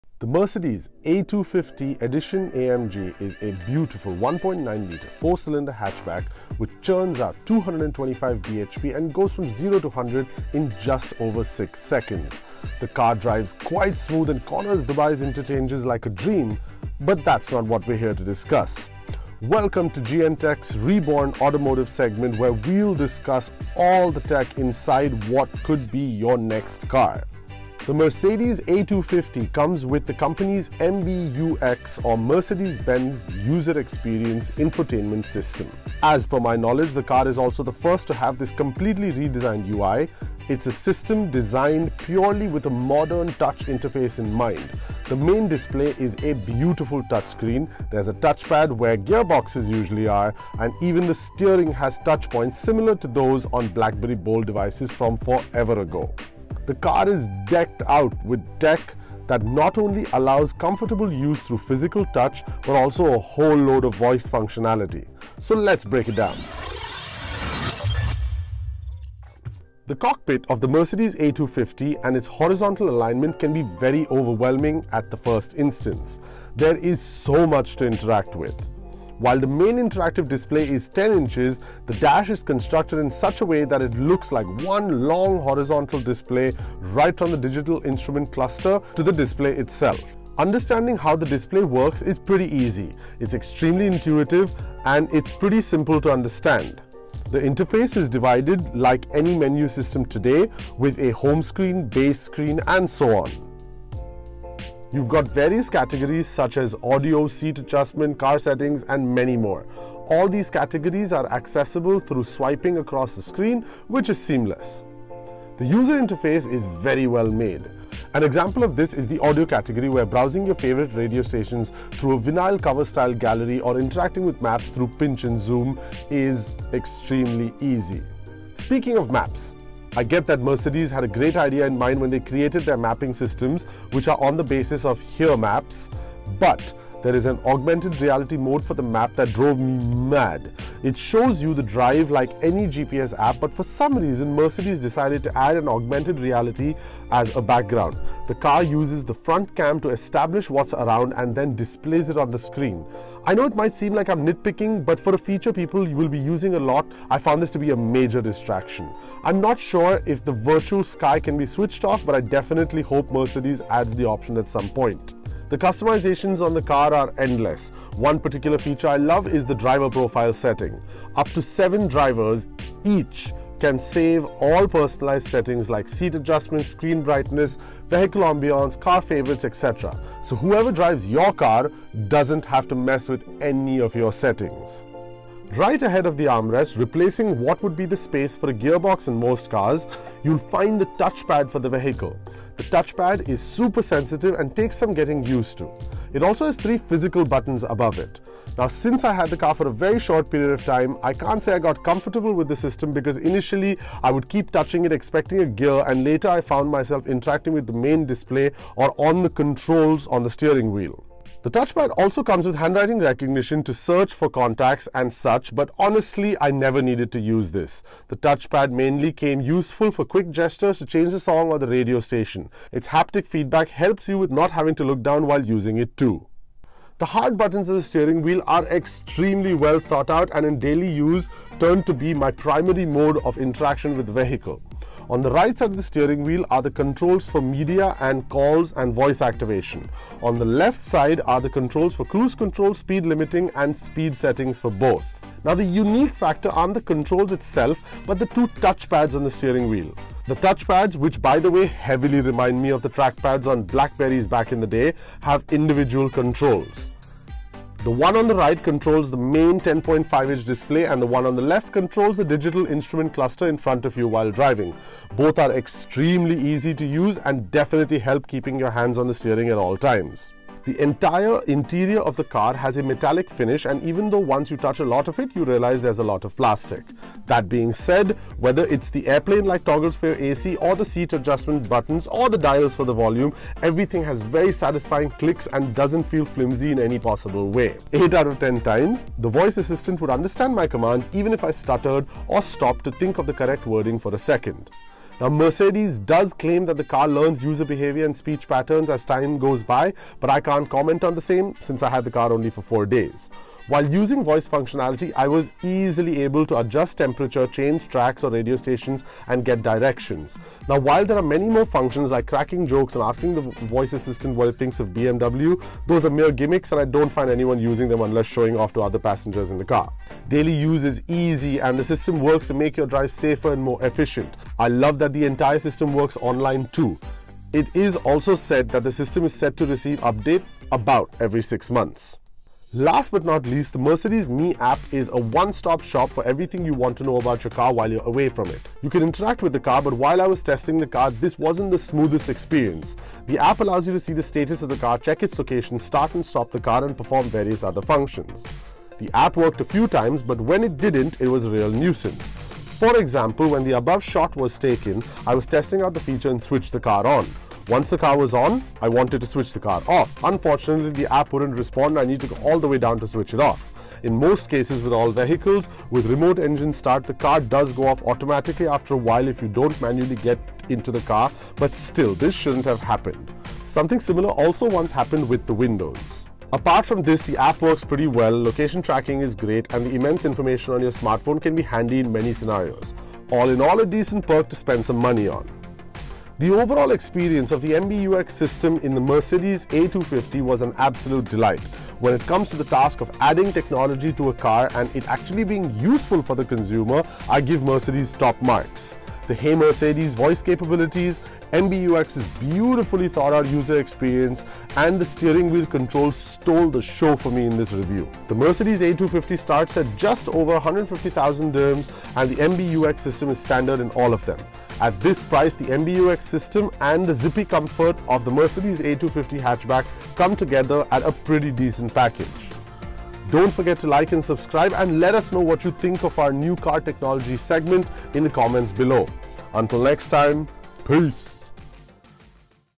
Mercedes Benz A Series - MBUX Review (2019)
Mercedes Benz A250 - MBUX (Infotainment System) Review

We're glad to have our automotive technology section back on GNTECH.ae. First in line is the Mercedes A250 and it's, newly locally launched, Mercedes Benz User Experience (MBUX) system. Here's everything you need to know about their most advanced voice and touch infotainment system in a Mercedes ever!

CREDITS

Voiceover and Edit:
Daanesh Kalyaniwalla - @daanyk55